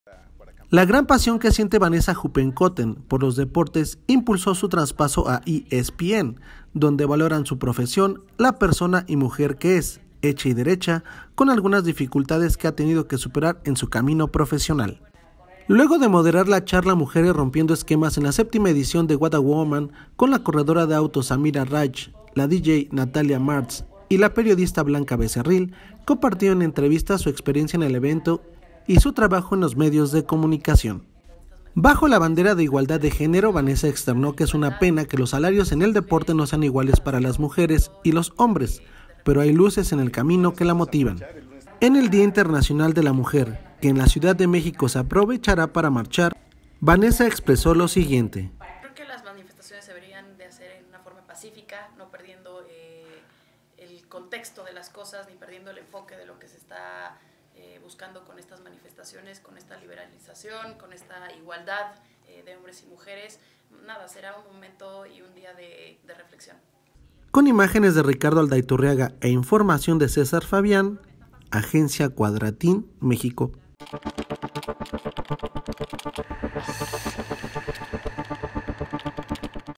Vanessa Huppenkothen, entre las mujeres rompiendo esquemas
La gran pasión que siente Vanessa Huppenkothen por los deportes impulsó su traspaso a ESPN, donde valoran su profesión, la persona y mujer que es, hecha y derecha con algunas dificultades que ha tenido que superar en su camino profesional.
Sus expectativas como presentadora del noticiario Sport Center se cumplen, pues buscaba una experiencia en un medio exclusivo de deportes, porque le apasionan y su llegada a El Líder Mundial en Deportes se tuvo que negociar con Televisa, pero si ningún problema se dio su "traspaso, como de futbolista (rió ligeramente)".
Luego de moderar la charla Mujeres Rompiendo Esquemas en la séptima edición de What a Woman con la corredora de autos Samira Rached, la dj Natalia Martz y la periodista Blanca Becerril, compartió en entrevista su experiencia en el evento y su trabajo en los medios de comunicación.